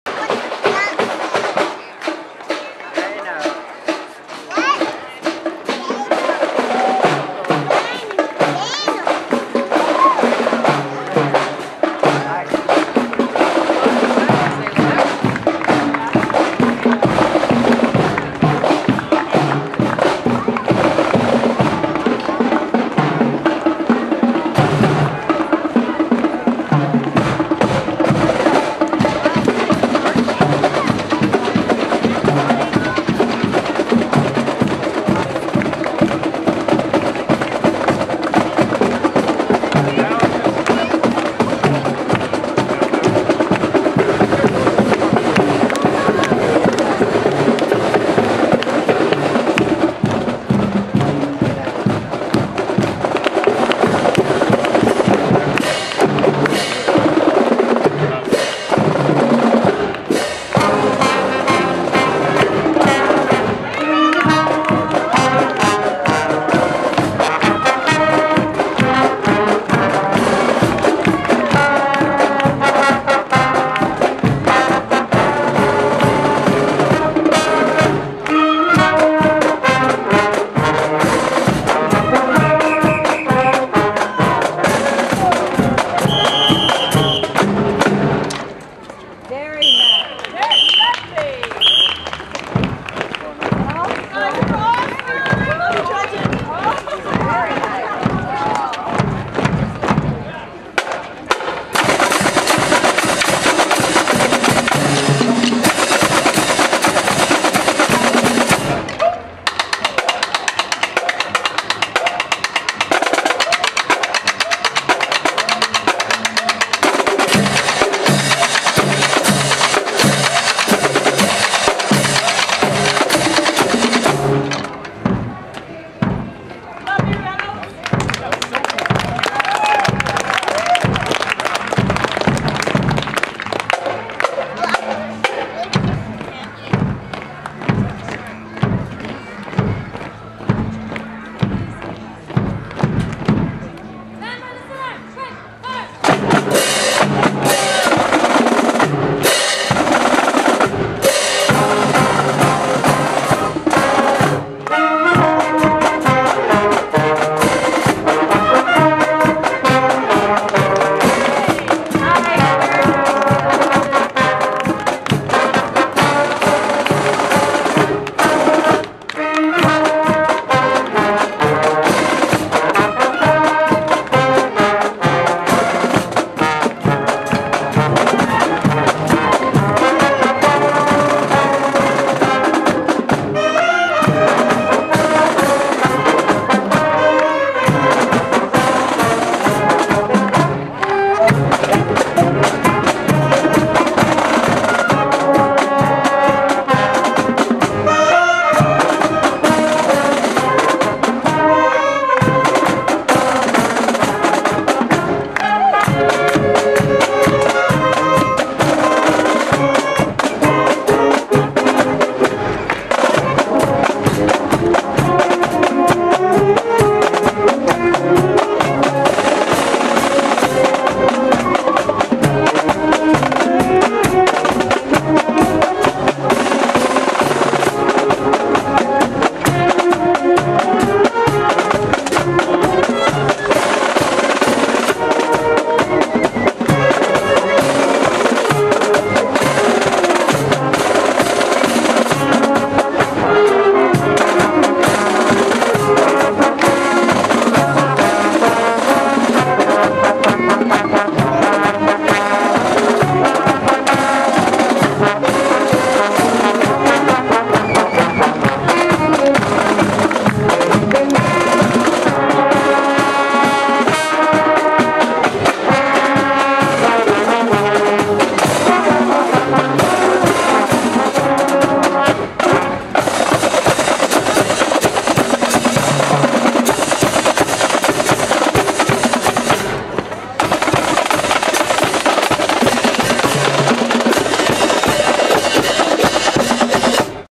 Reynolds Marching Band (Vid#2) in 2014 Oak Bay Tea Party Parade
Second video of the Reynolds Marching Band in Victoria's Oak Bay Tea Party Parade, June 7, 2014.

0:54 - Music starts at
1:30 - Special percussion performance for Reynolds' past Principal.